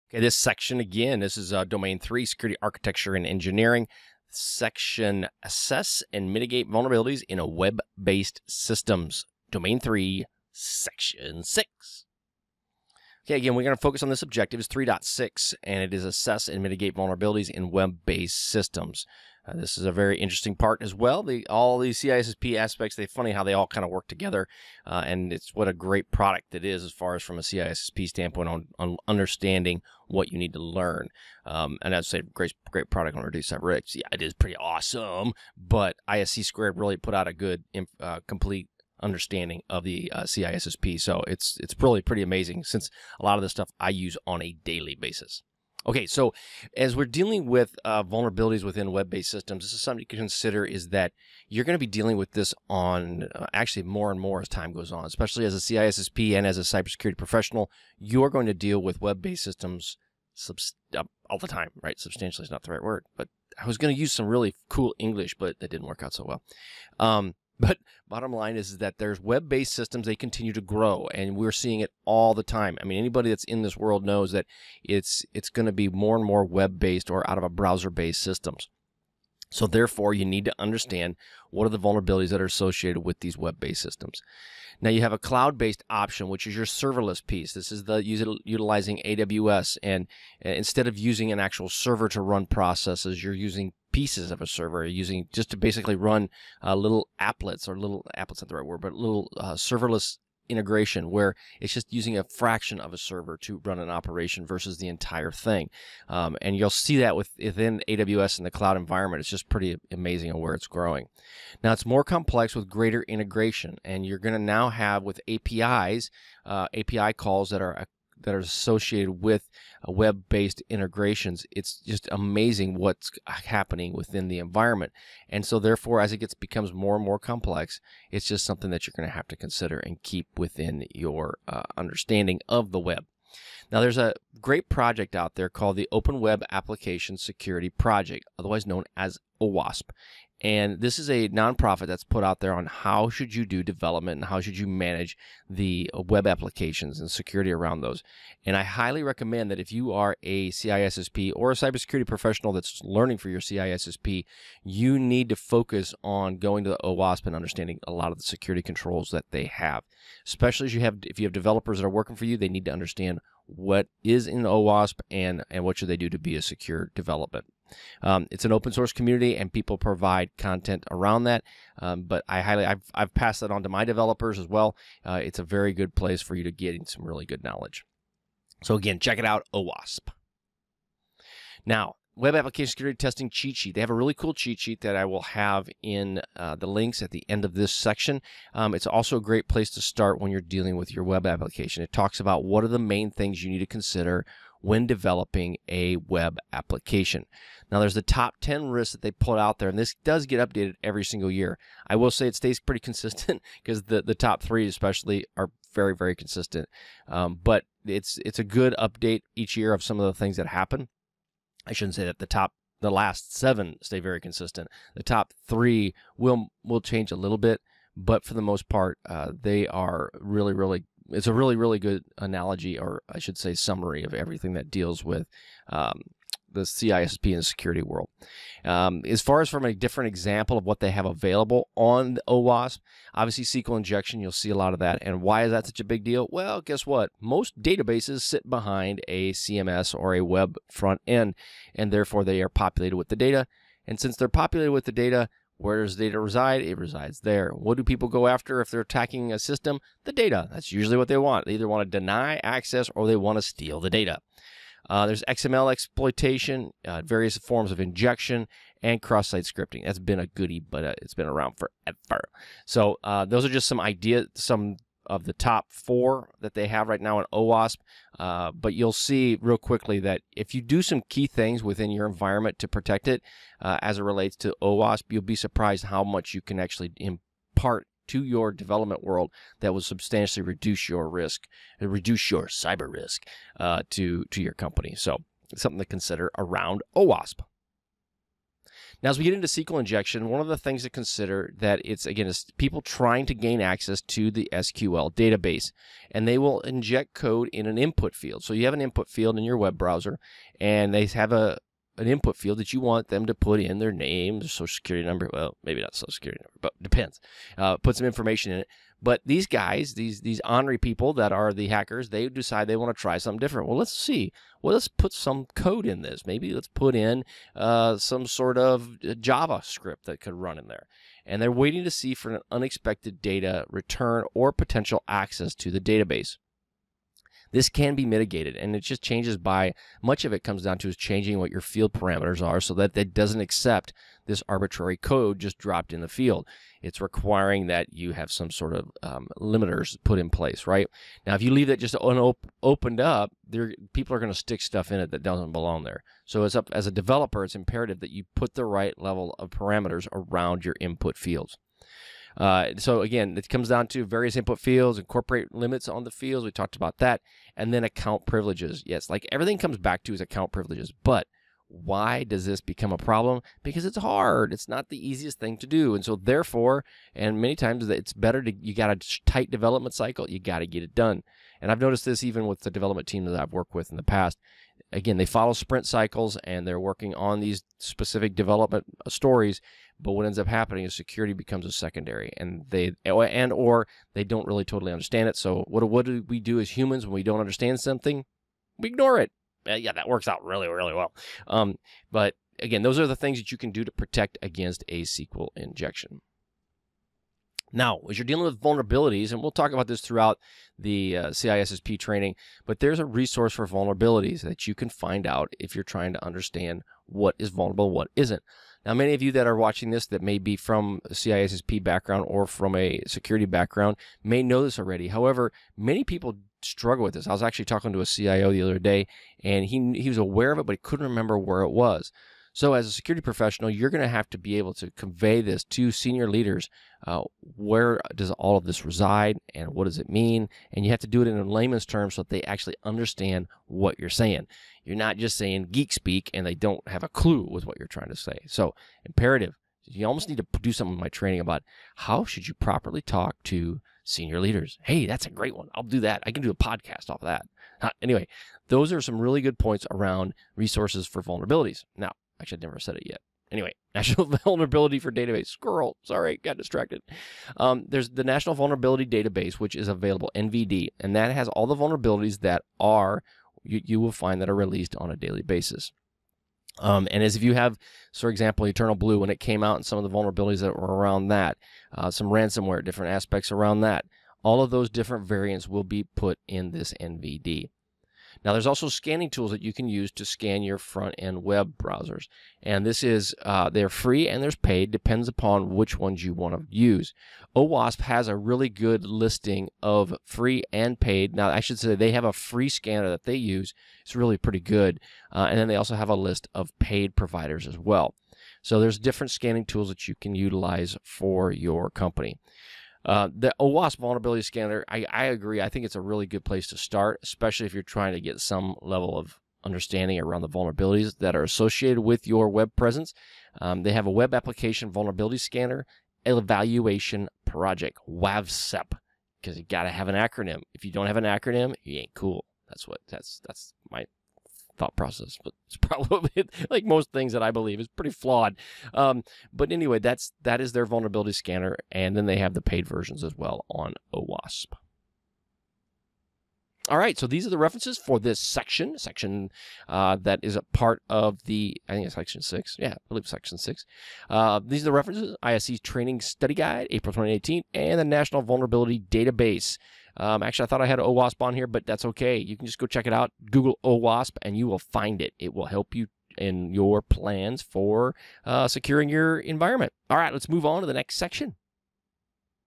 Domain 3 - Assess and Mitigate Vulnerabilities
In this video, you will learn how to assess and mitigate vulnerabilities to pass the CISSP Exam.